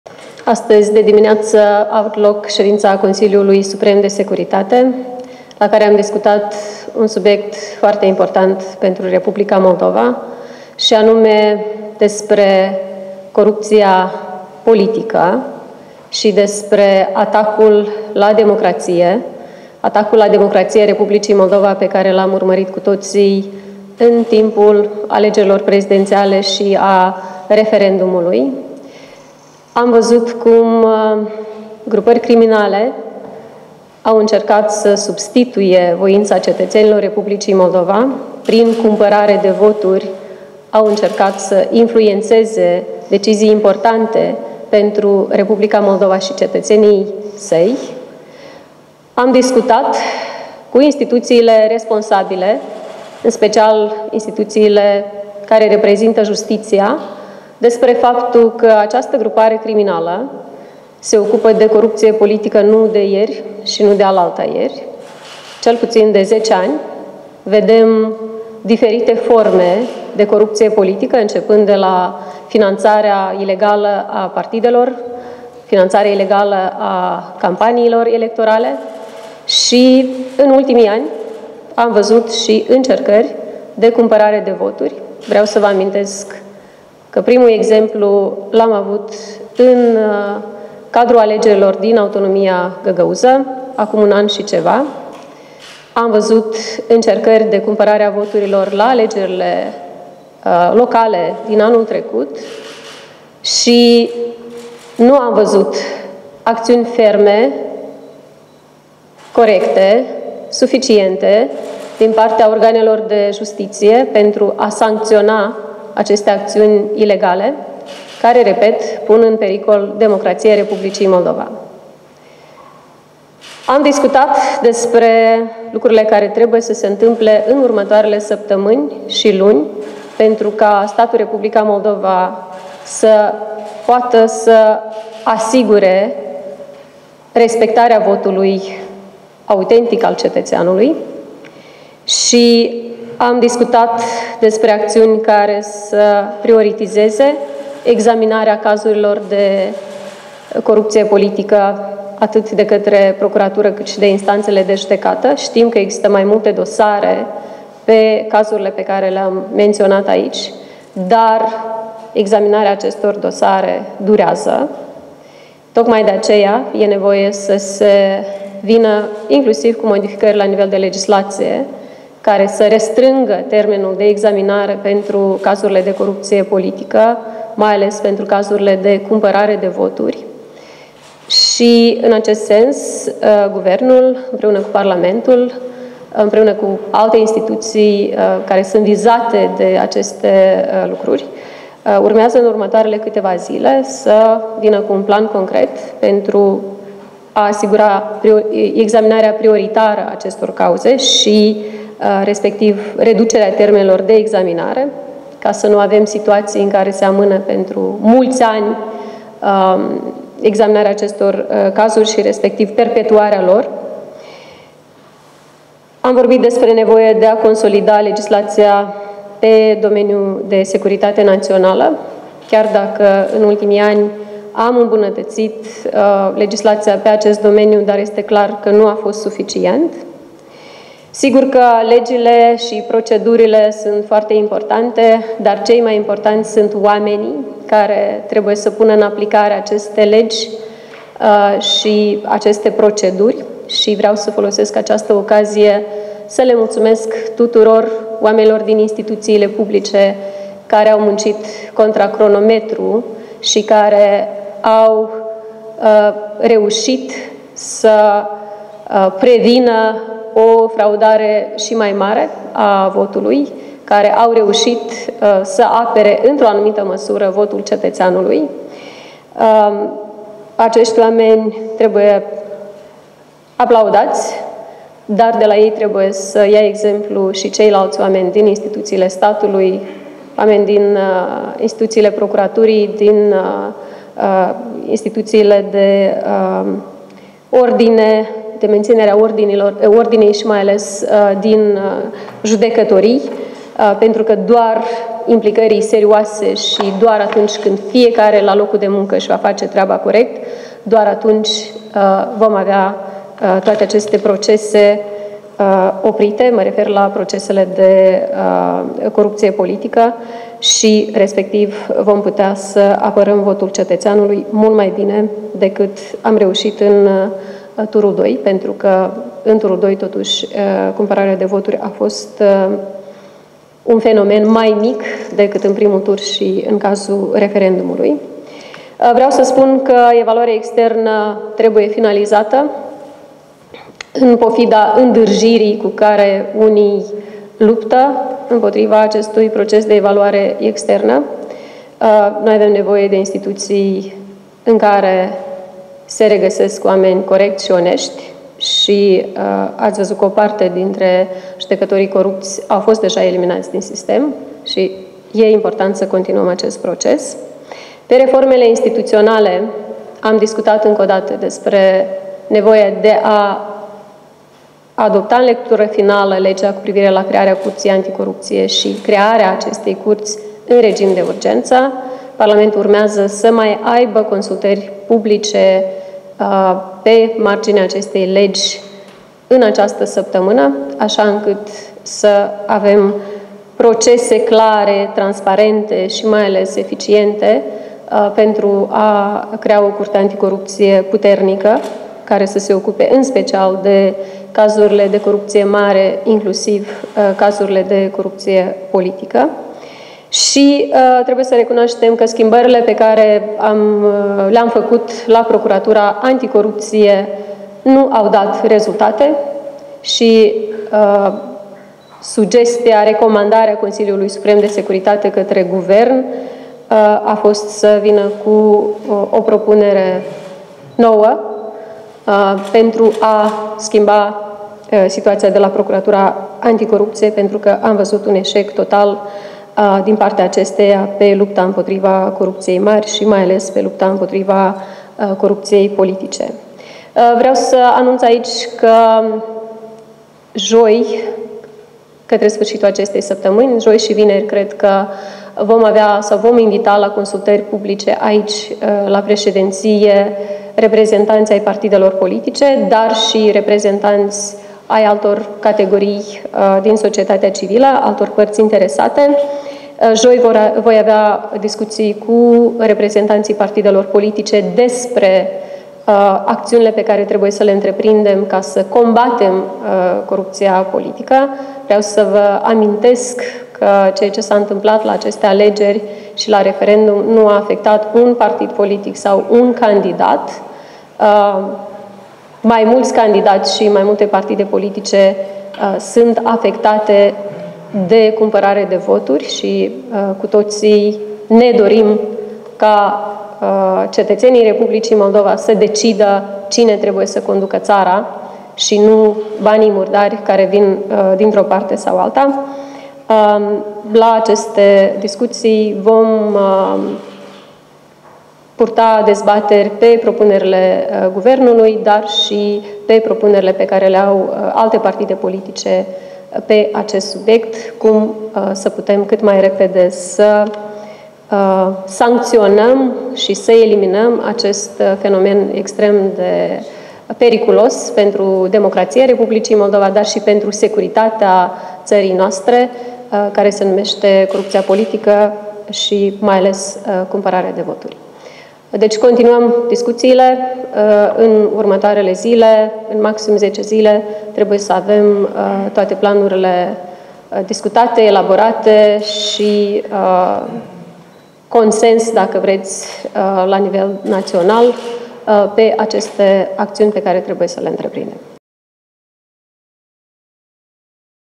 Declarațiile șefei statului după ședința Consiliului Suprem de Securitate
Președinta Maia Sandu a solicitat Consiliului Suprem de Securitate acțiuni ferme și rapide pentru combaterea corupției politice